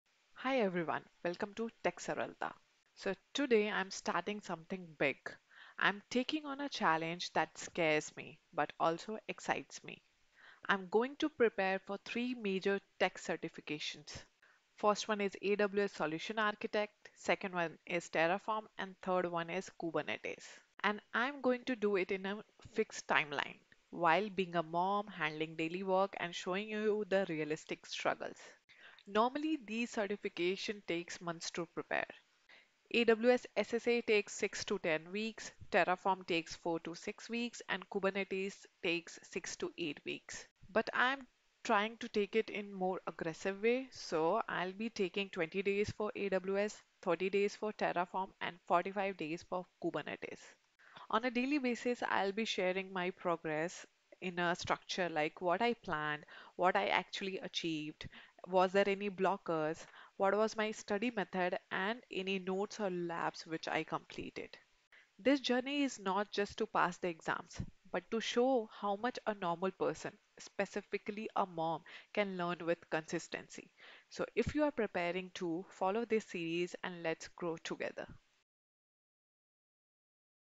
Can I Clear 3 Tech Certifications in 100 Days? 😳 Let’s Find Out!
Welcome to  100-Day Cloud Certification Challenge!
I’m attempting THREE major certifications back-to-back:
🌩 AWS Solutions Architect Associate – 20 Days
🧱 Terraform Associate – 30 Days
☸ Kubernetes CKA – 45 Days

Every single day, I’ll share:
✨ My study plan
📚 What I actually completed
⏳ How much time I studied
⚠️ Blockers, struggles & mindset
💡 Realistic progress – no sugar-coating